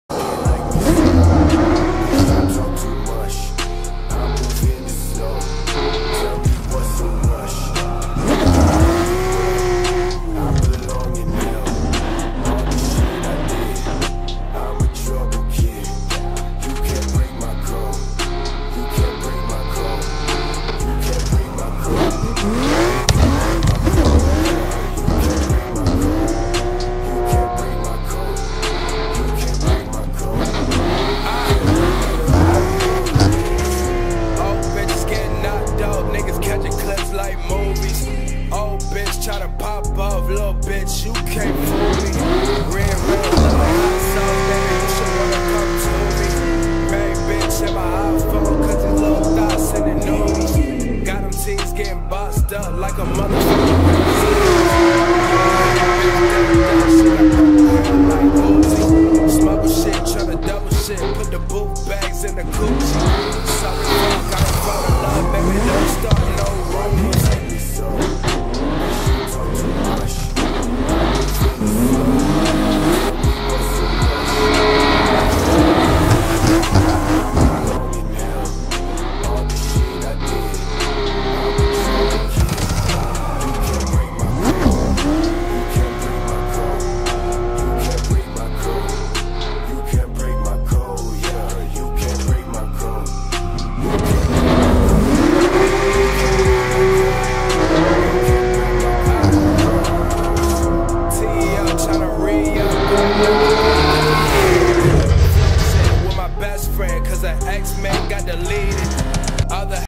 ASBO SUPRA HITS LAGUNA SECA
TESTING OUT MY NEW ASBO SUPRA GR FD